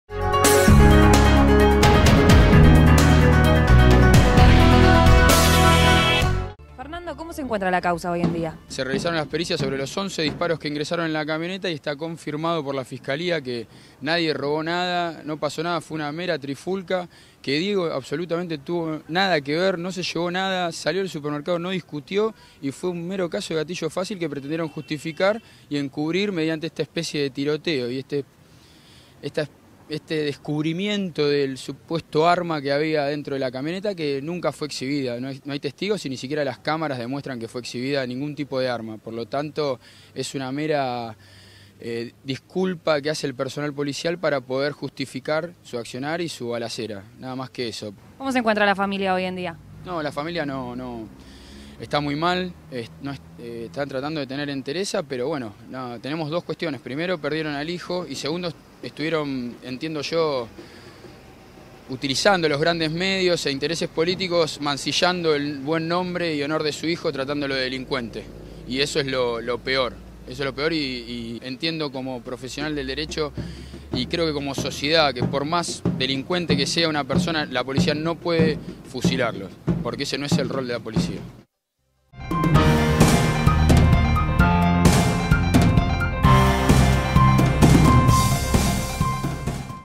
Joven asesinado en Martín Coronado: “Está confirmado que no hubo ningún robo”, aseguran
Fernando Sicilia, abogado de la familia Cagliero, señaló a este medio: “Diego no se robó nada, no discutió y fue un caso de gatillo fácil que pretendieron encubrir con ese tiroteo”.